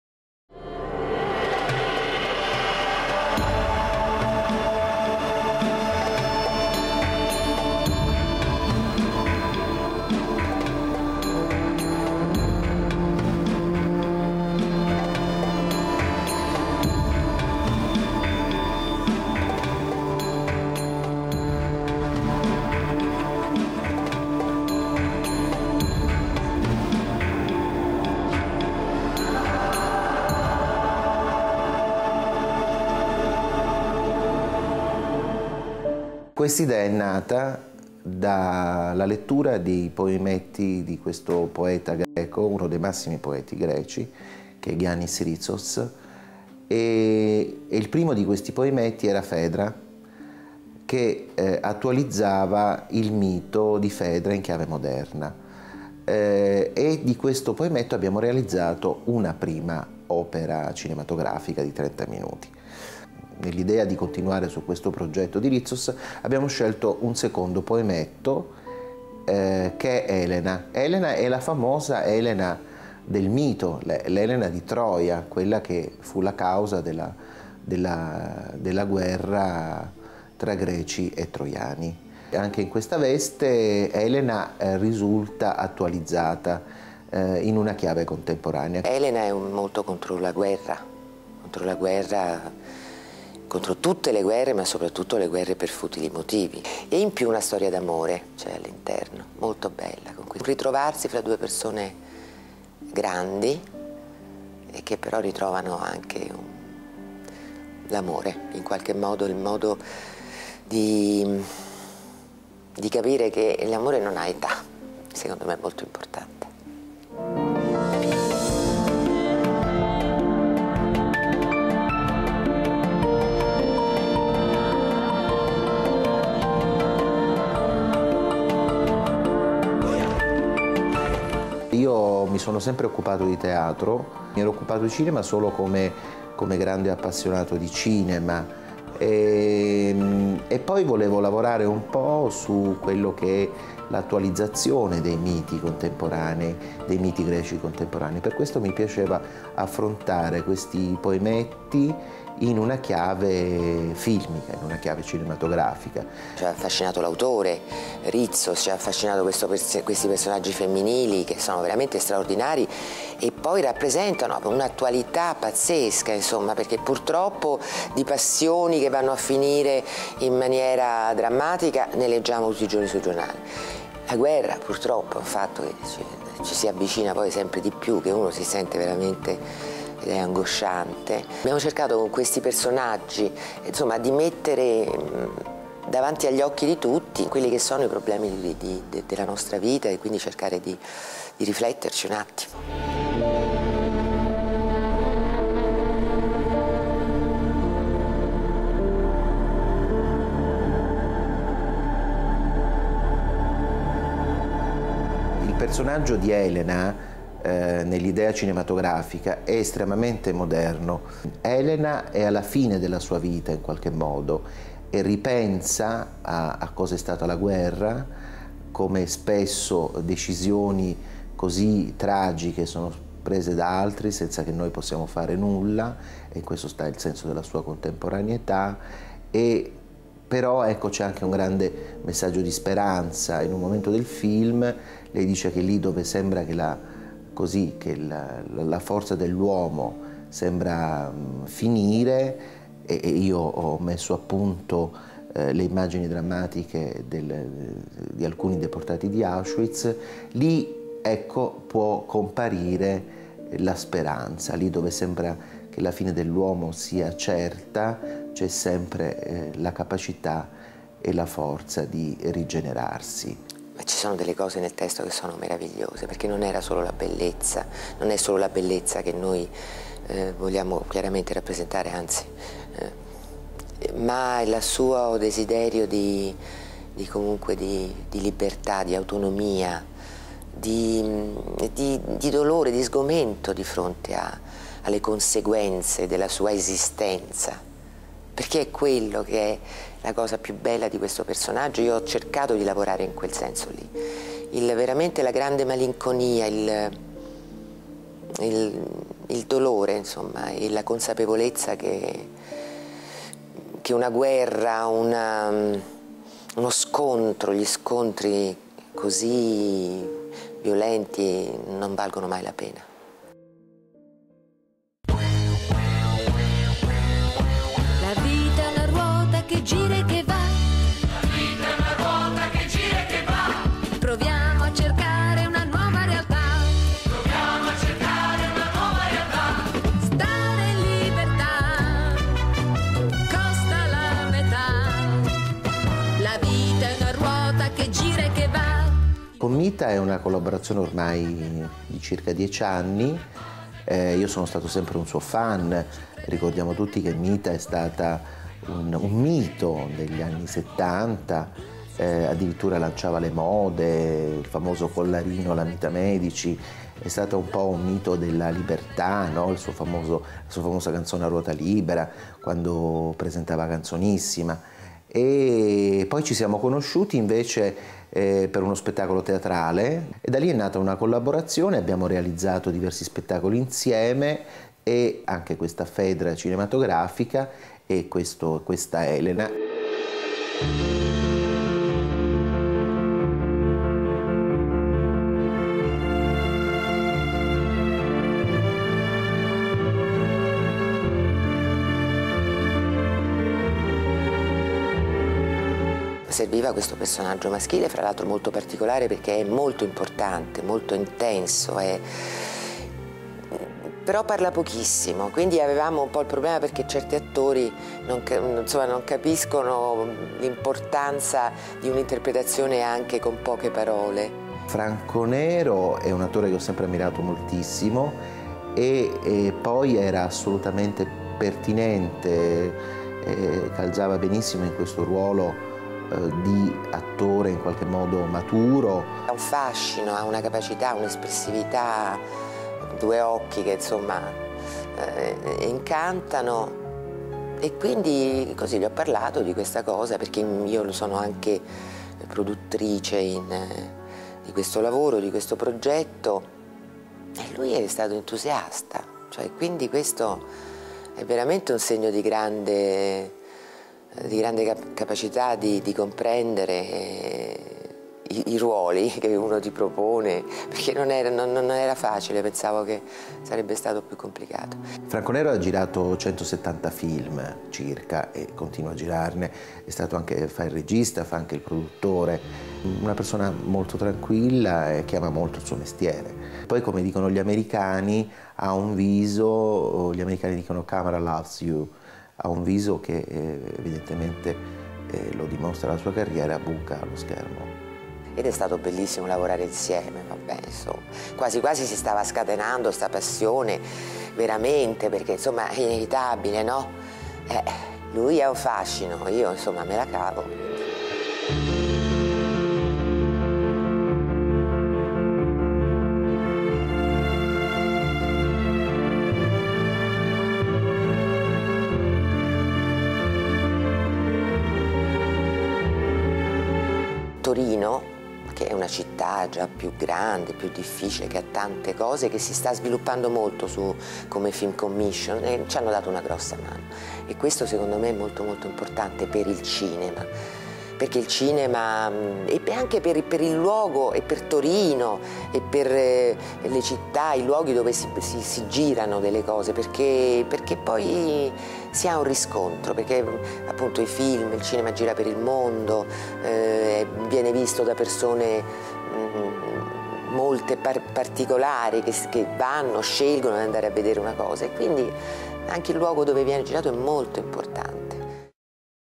Il Terzo Fiore - making of "Elena" - part 01
Il backstage del cortometraggio "Elena", di Salvo Bitonti, con Franco Nero e Mita Medici.
Un documentario di Alessandro Righetti.
- prima parte -